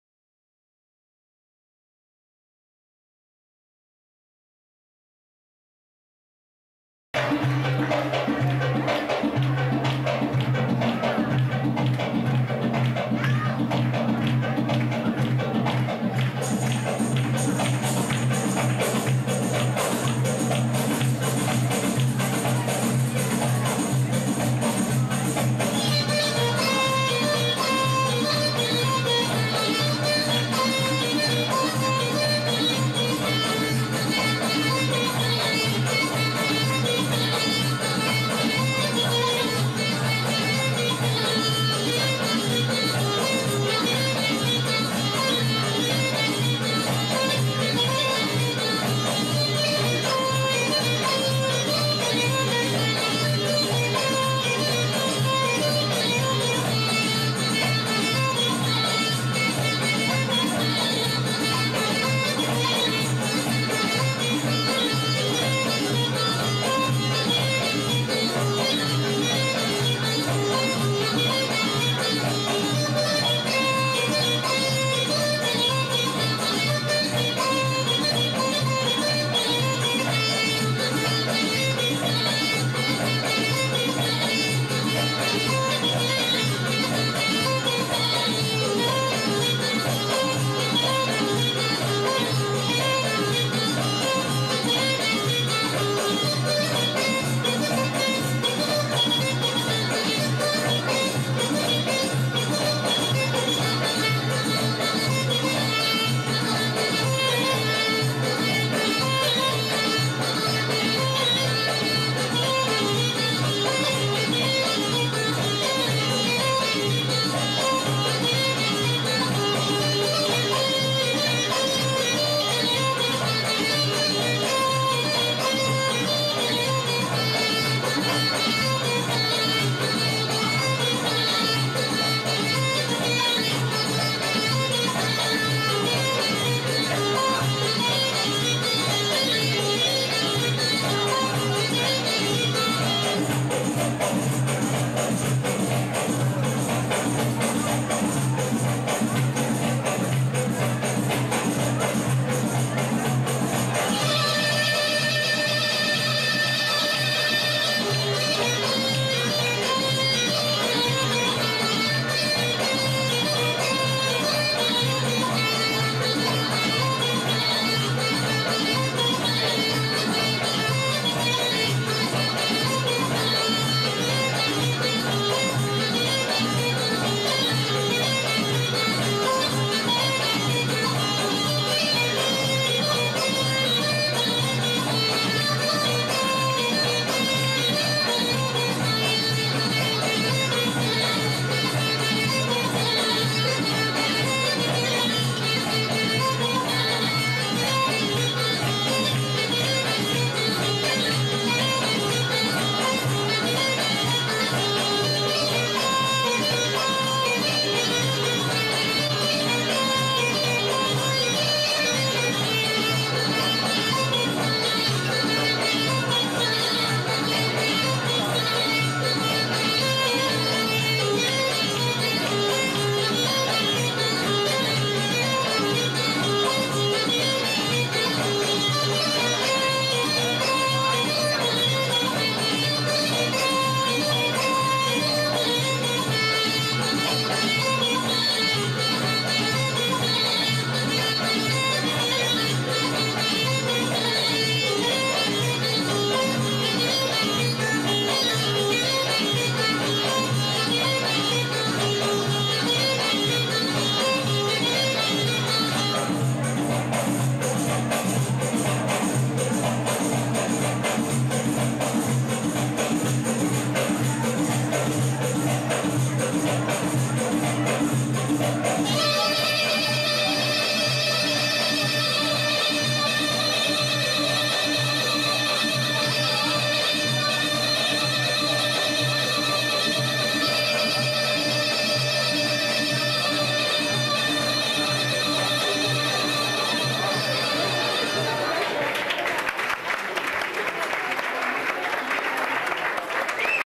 Persian Bandari Dance for Norooz - Caspian, 2009
This dance was part of a performance for Norooz at the Caspian in Irvine, CA in 2009. The Bandari dance was primarily choreographed by Miranda, with contributions from Lilla Varese and Joulya. The music is very traditional. The jackets were made by Lilla and the skirts and blouses were part of Miranda's costume collection. The intention was to achieve a traditional Persian look as best as possible with little time and a small budget. We were fairly happy with how it turned out, though we could have used a touch more rehearsal before the show.